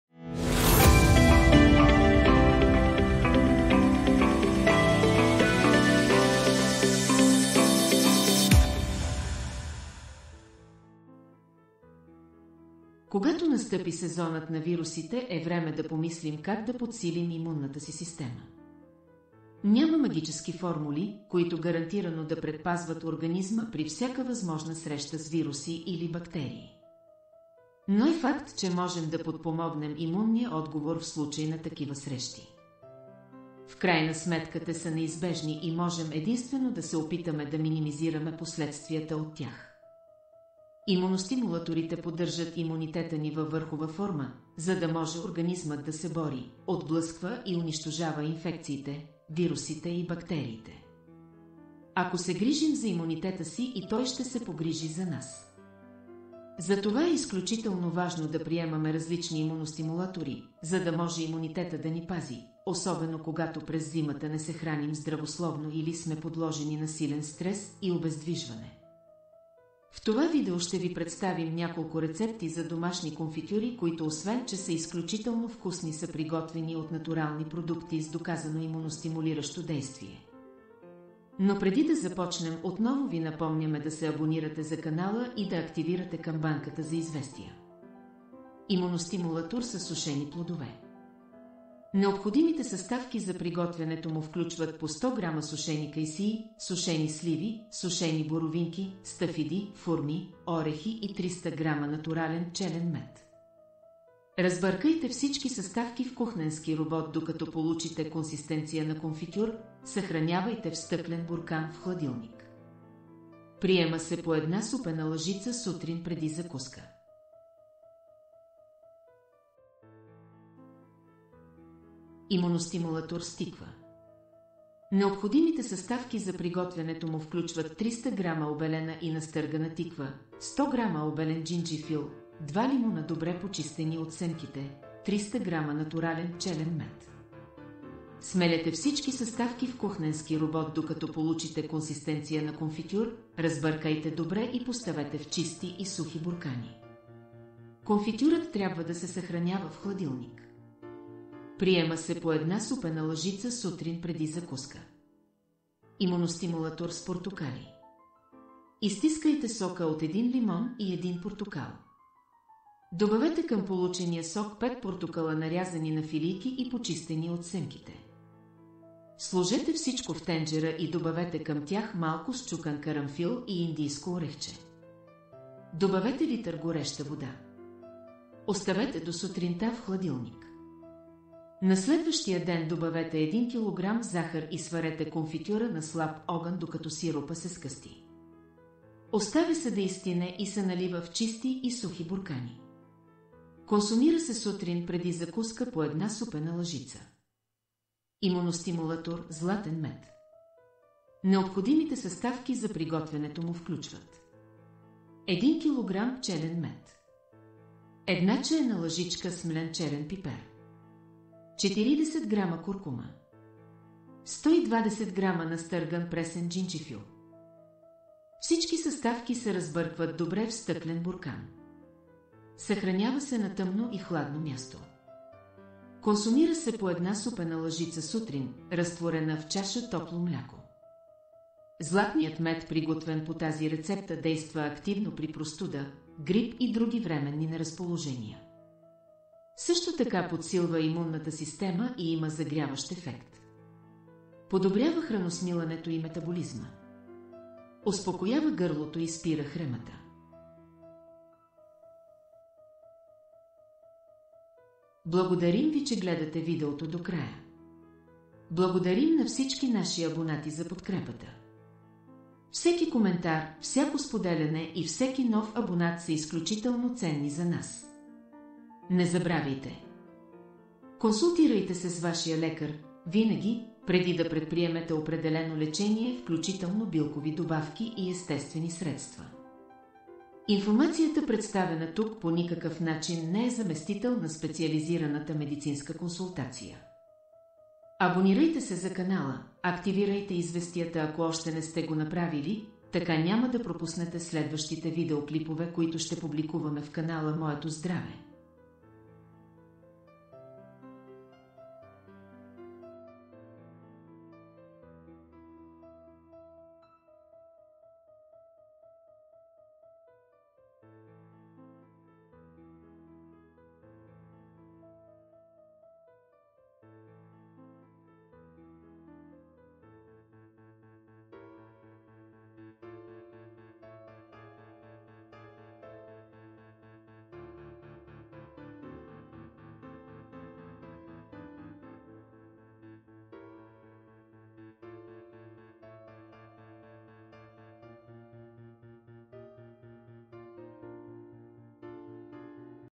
Защитете се от болести с тези 4 рецепти за конфитюр, които укрепват имунната система!
Когато настъпи сезонът на вирусите е време да помислим как да подсилим имунната си система. Няма магически формули, които гарантирано да предпазват организма при всяка възможна среща с вируси или бактерии. Но е факт, че можем да подпомогнем имунния отговор в случай на такива срещи. В крайна сметка те са неизбежни и можем единствено да се опитаме да минимизираме последствията от тях.
Имуностимулаторите поддържат имунитета ни във върхова форма, за да може организмът да се бори, отблъсква и унищожава инфекциите, вирусите и бактериите. Ако се грижим за имунитета си и той ще се погрижи за нас. Затова е изключително важно да приемаме различни имуностимулатори, за да може имунитета да ни пази, особено когато  през зимата не се храним здравословно или сме подложени на силен стрес и обездвижване.
В това видео ще ви представим няколко рецепти за домашни конфитюри които освен че са изключително вкусни са приготвени от натурални продукти с доказано имуностимулиращо действие. Но преди да започнем отново ви напомняме да се абонирате за канала и да активирате камбанката за известия.

Имуностимулатор със сушени плодове.
Необходимите съставки за приготвянето му включват по 100 грама сушени кайсии, сушени сливи, сушени боровинки, стафиди, фурми, орехи и 300 грама натурален пчелен мед. Разбъркайте всички съставки в кухненски робот докато получите консистенция на конфитюр, съхранявайте в стъклен буркан в хладилник. Приема се по една супена лъжица сутрин преди закуска.

Имуностимулатор с тиква.
Необходимите съставки за приготвянето му включват 300 грама обелена и настъргана тиква, 100 грама обелен джинджифил, два лимона добре почистени от семките, 300 грама натурален пчелен мед. Смелете всички съставки в кухненски робот докато получите консистенция на конфитюр, разбъркайте добре и поставете в чисти и сухи буркани. Конфитюрът трябва да се съхранява в хладилник. Приема се по една супена лъжица сутрин преди закуска.

Имуностимулатор с портокали.
Изстискайте сока от един лимон и един портокал. Добавете към получения сок пет портокала нарязани на филийки и почистени от семките. Сложете всичко в тенджера и добавете към тях малко счукан карамфил и индийско орехче. Добавете литър гореща вода. Оставете до сутринта в хладилник. На следващия ден добавете един килограм захар и сварете конфитюра на слаб огън докато сиропа се сгъсти. Оставя се да изстине и се налива в чисти и сухи буркани. Консумира се сутрин преди закуска по една супена лъжица.

Имуностимулатор "Златен Мед"
Необходимите съставки за приготвянето му включват
Един килограм пчелен мед.
Една чаена лъжичка смлян черен пипер.
40 грама куркума.
120 грама настърган пресен джинджифил.

Всички съставки се разбъркват добре в стъклен буркан. Съхранява се на тъмно и хладно място. Консумира се по една супена лъжица сутрин, разтворена в чаша топло мляко. Златният мед приготвен по тази рецепта действа активно при простуда, грип и други временни неразположения. Също така подсилва имунната система и има загряващ ефект. Подобрява храносмилането и метаболизма. Успокоява гърлото и спира хремата.